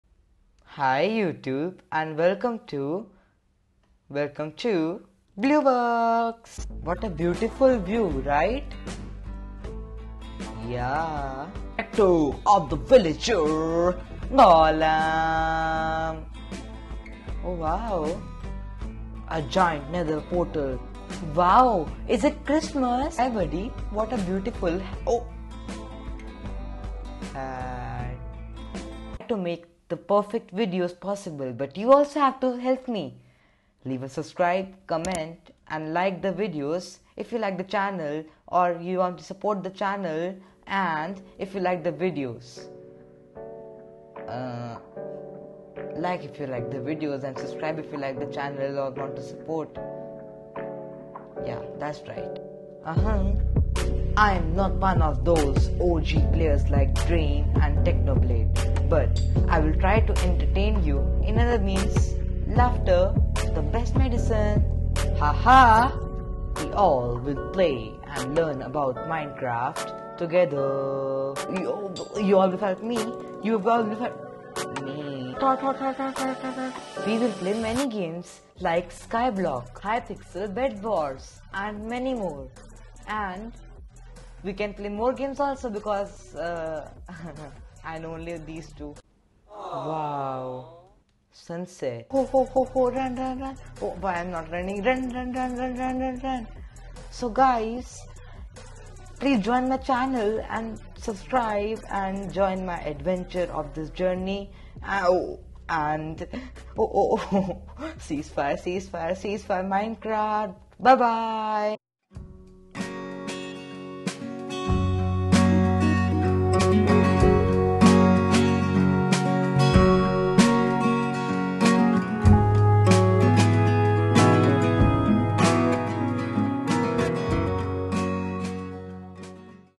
Meet Me
Hi YouTube
I am Blueworks
I hope you all found it entertaining, please join my journey of this big mountain.     YOUTUBE
I know it would sound strange to call me always during the vids
BLUEWORKS BLUEWORKS BLUEWORKS
So I created a new nickname
Works.       I Will call myself as WORKS
NICKNAME
Thanks Guys to watch this Video